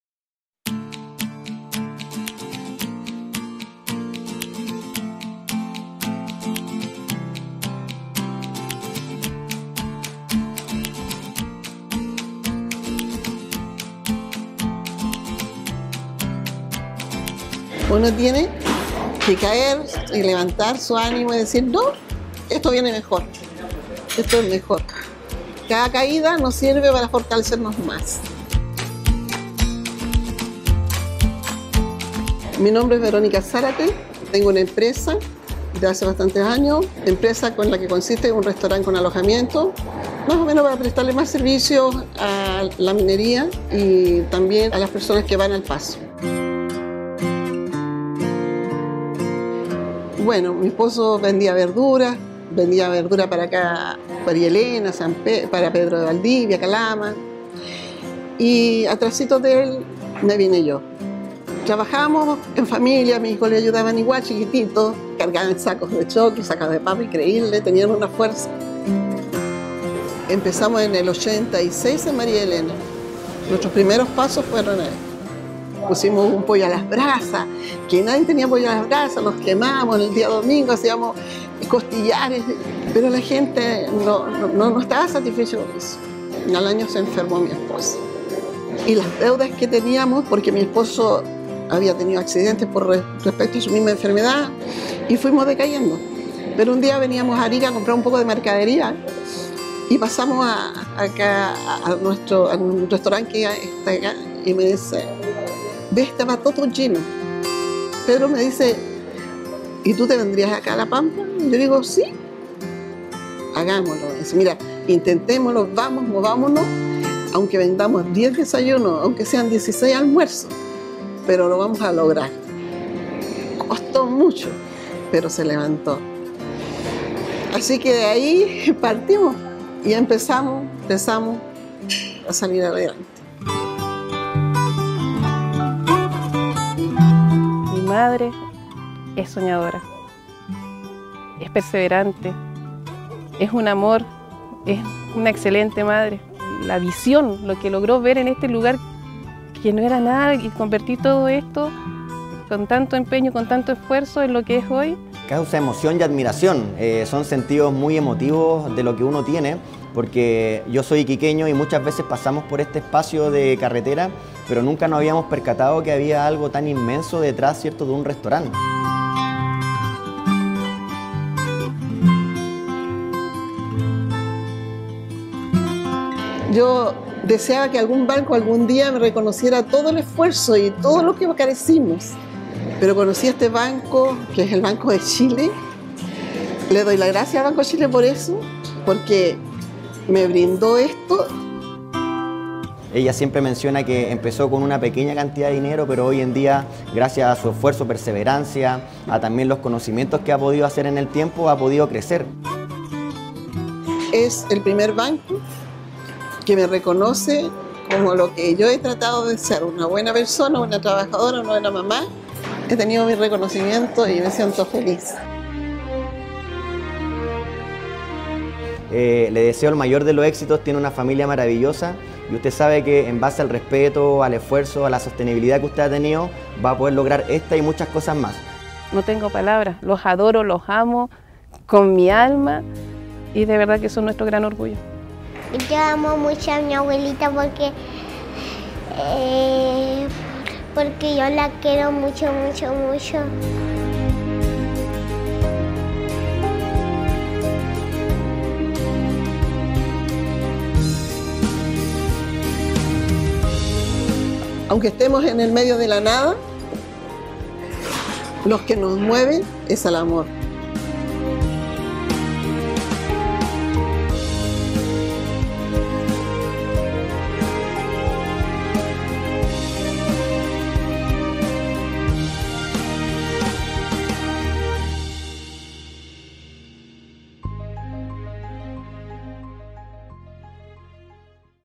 Banco de Chile - Historias PYME Veronica Zárate
Verónica Zárate // Empresa: Restaurante San Roque // Rubro: Restaurante y Servicio de Hospedaje